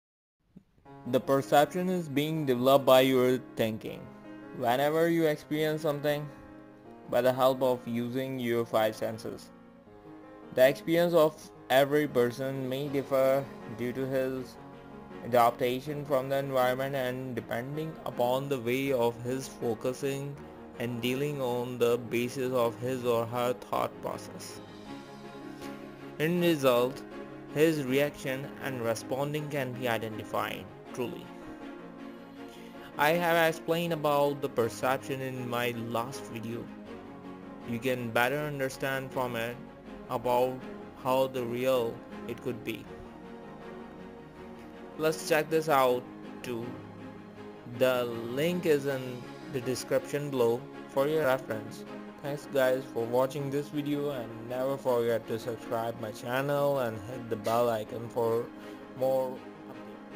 Motivation call for learning!| Basis of Developing Perception | Experiences may differs
The Perception is being developed by your thinking. Whenever you Experience something, by the help of using your five senses
The experience of every person may differ due to his adaptation from the environment and depending upon the way of his focusing and dealing on the basis of his or her thought process. In results his reaction and responding can be identified. I have explained about the Perception in my last video, you can better understand from it about how real it could be  let’s check this out too… the link is here below for your reference.

link of my previous video